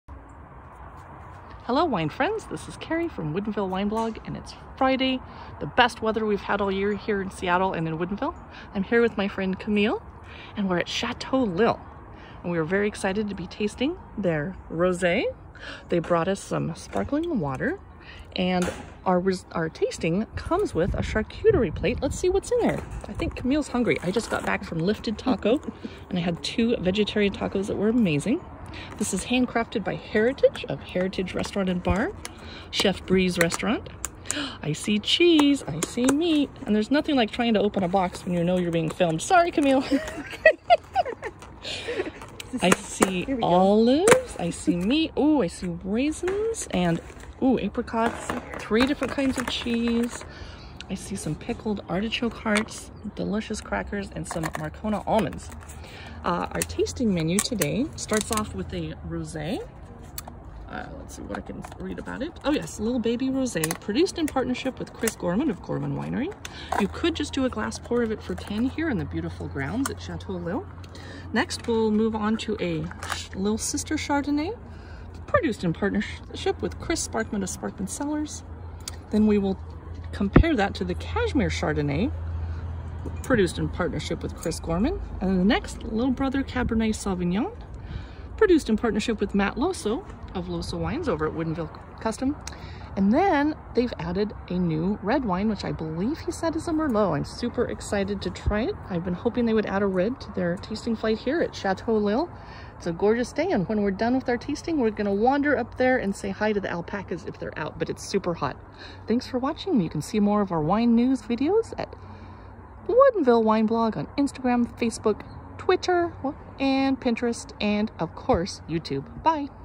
Tasting Wine and Nibbling on Charcuterie at Chateau Lill
I recently brought my friend Camille to Chateau Lill in Woodinville for wine tasting. We were provided with a charcuterie box by Heritage Restaurant | Bar, with nuts, raisins, apricots, pickled artichoke hearts, and three kinds of cheese.

Our tasting menu started with Lill Bebe Rose, then Lill Sister Chardonnay, then Cashmere Chardonnay. Next to the reds - Lill Brother Cabernet Sauvignon and the new Lill Brother Merlot.